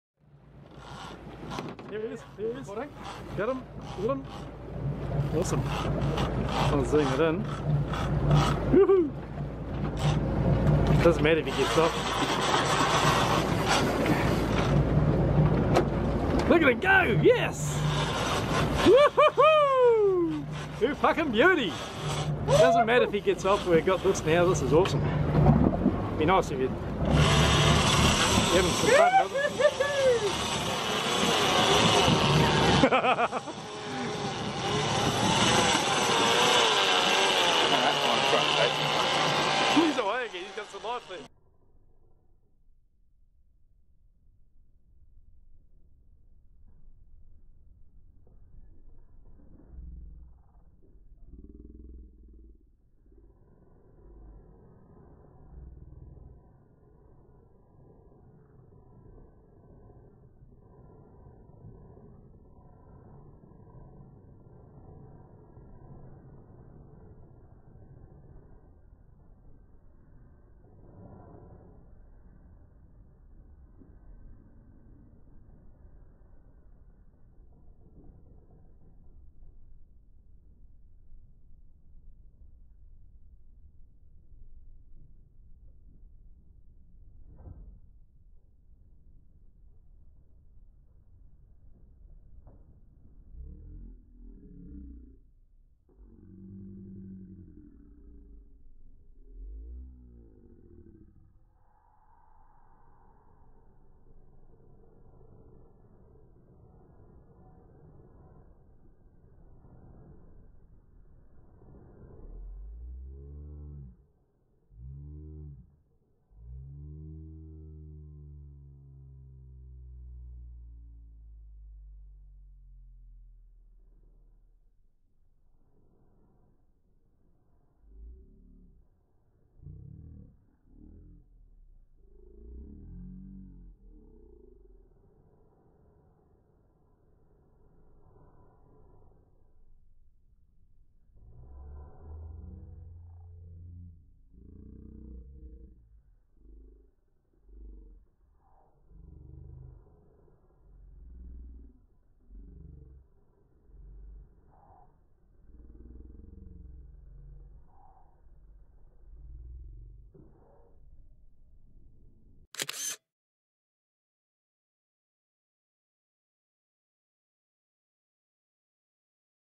Marlin Aerial Display
Striped Marlin going mental, Daiwa 50w screaming. Warning this video contains language which may offend some people.